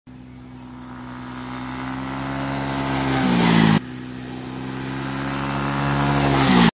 Trying to take curves like a pro NMAX155
Trying and pretending to curve a motorbike at the speed of 60kph in semi curve highway